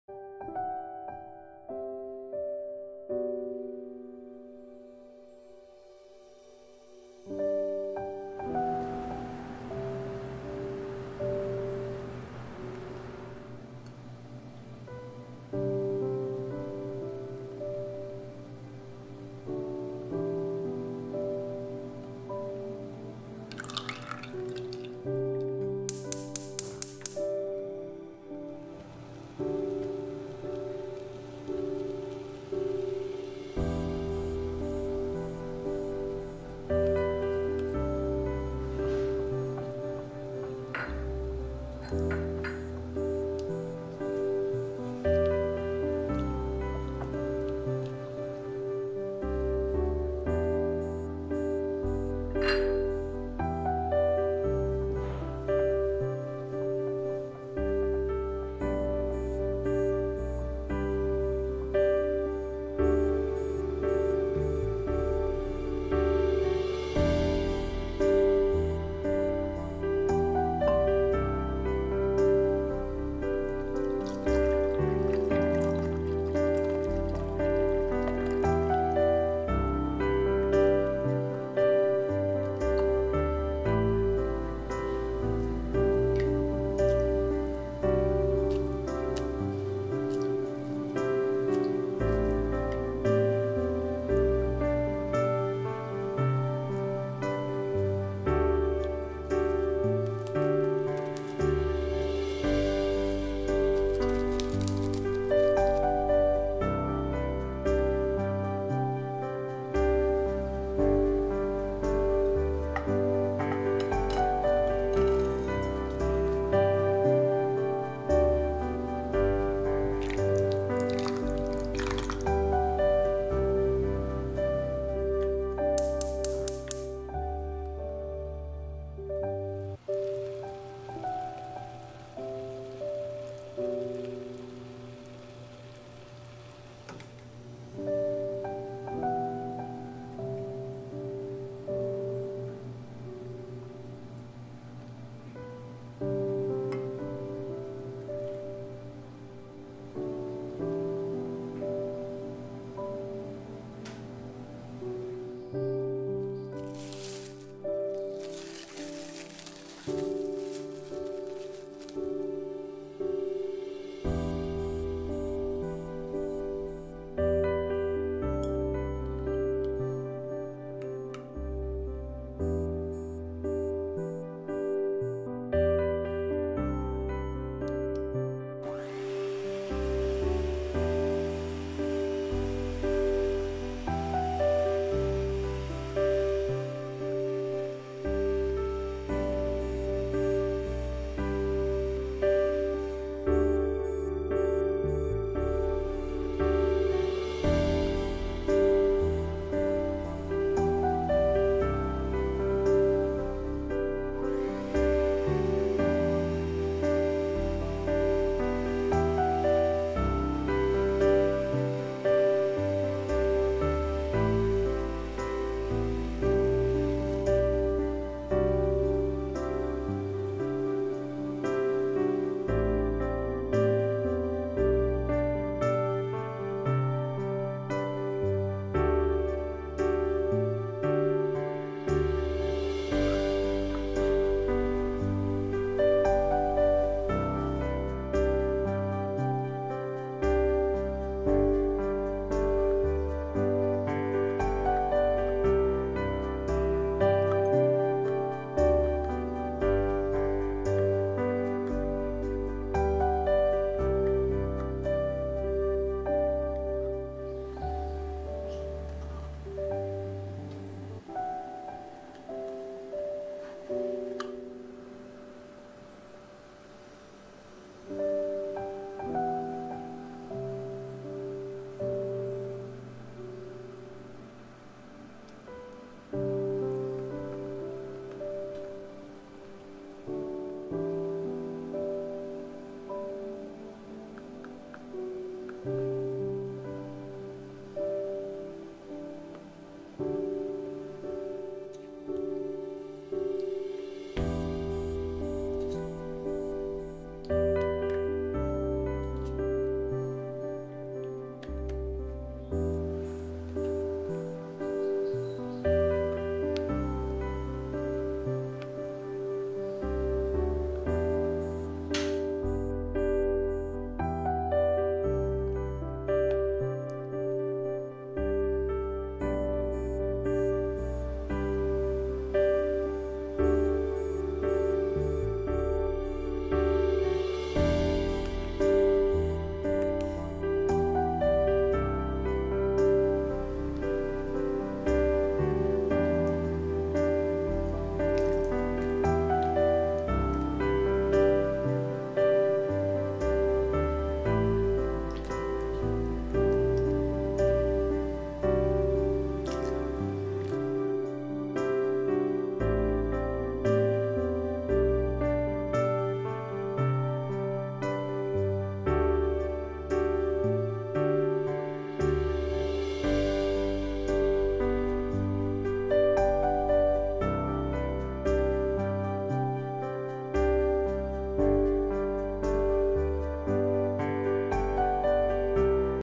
Kem Trà Sữa Đường Đen | Brown Sugar Milk Tea Ice Cream | The Sweetest Things
-Nguyên liệu:
  500ml kem tươi
  8g trà earl gray
  95ml-110ml sữa đặc
  80g đường nâu
  45ml nước
  Trân châu

-Ingredients:
  500ml cream
  8g earl grey tea
  95ml-110ml condensed milk
  80g brown sugar
  45ml water
  Boba pearls

From The Sweetest Things with Love and Passion